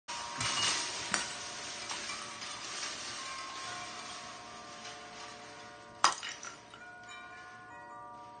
GISH 2018 # 38 Household Wind Chime
Create a stunning wind chime using unique household items.